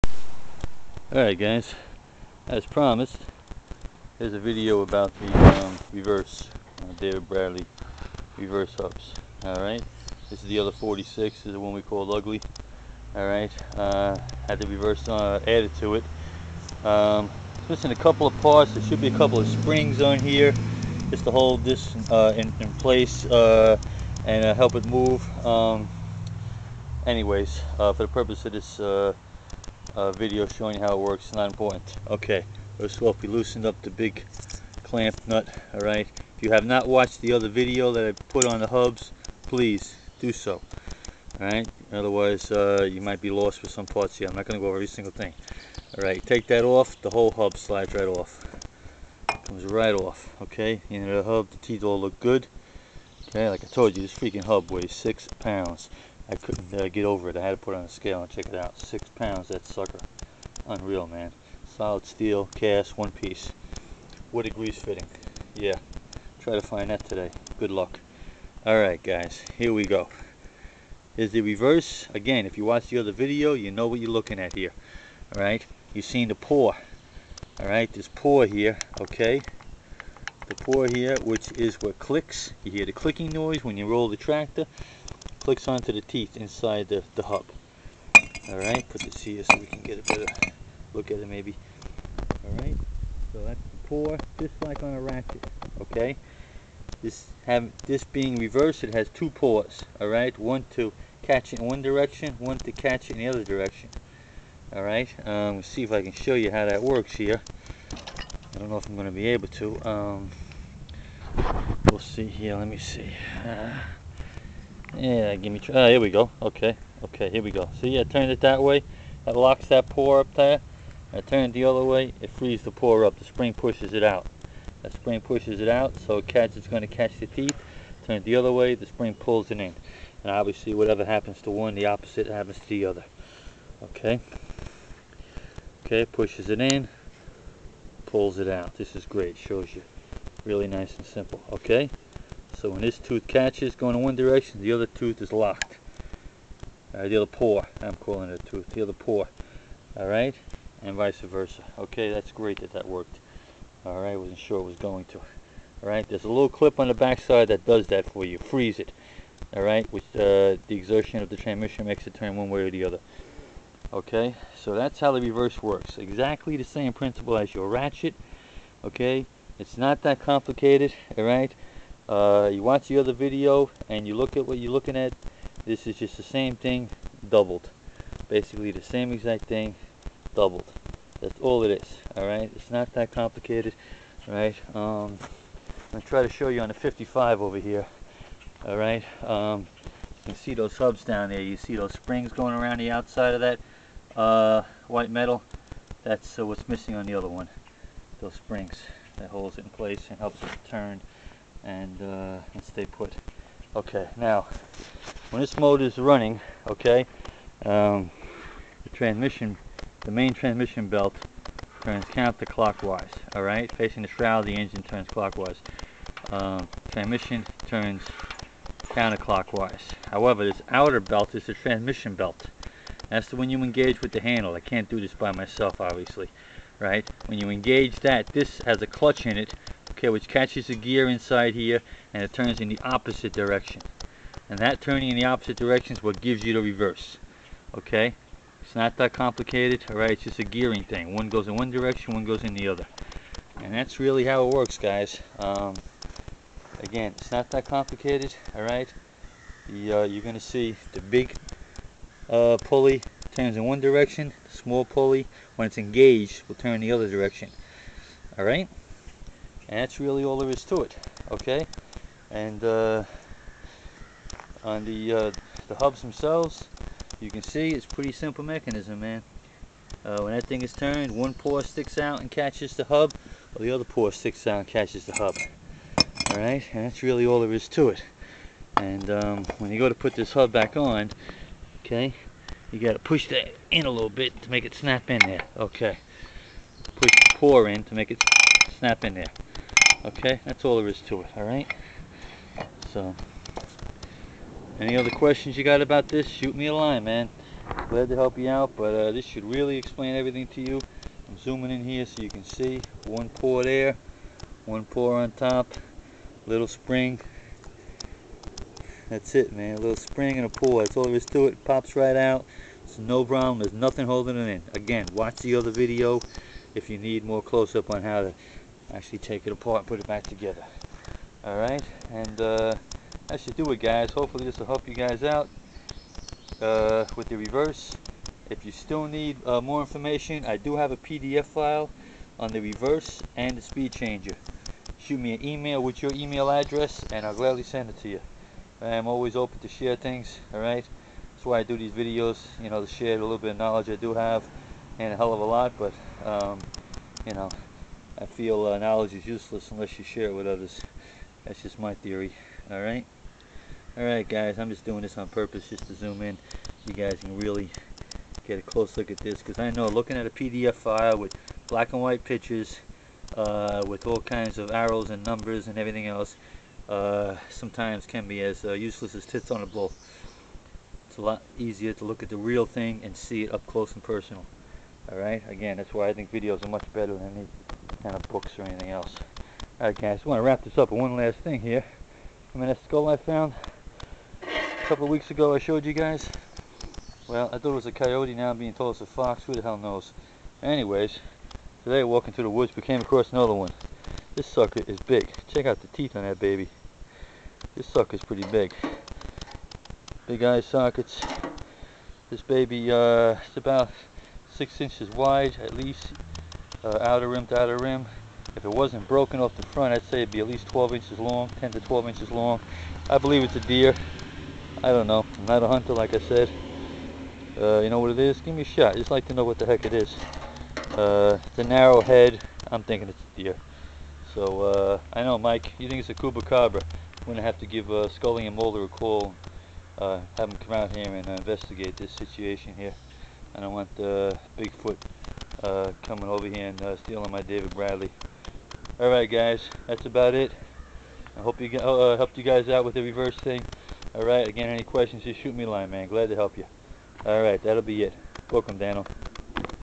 How David Bradley Tractor Reverse Hub Mechanism Works.
Up close Tear Down & Explanation  of  How the Reverse Hub Mechanism of the David Bradley Tractor Work